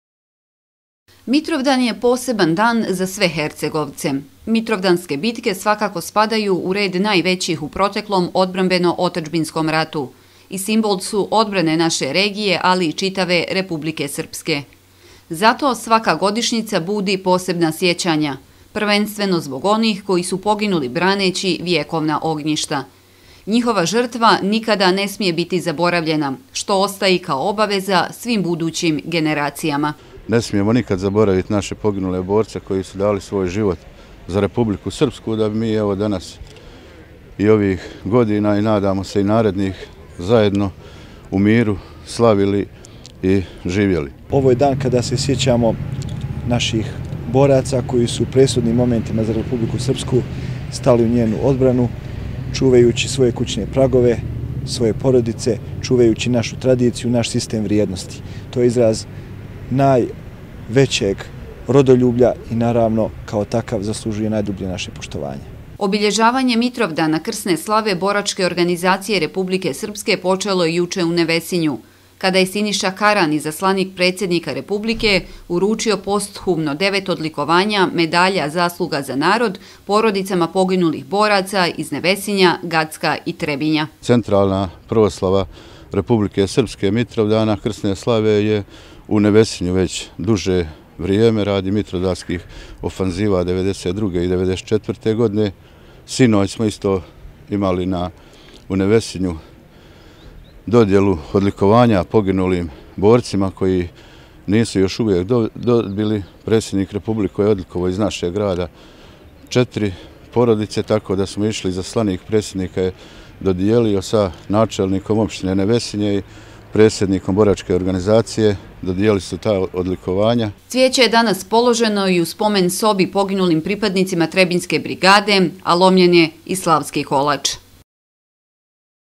Trebinje: Boračka organizacija obilježila Mitrovdan (08.11.2018.)
Polaganjem cvijeća na Spomenik braniocima Trebinja i pomenom poginulim borcima u Crkvi Svetog cara Lazara i Boračka organizacija Trebinje obilježila je krsnu slavu Boračke organizacije Republike Srpske.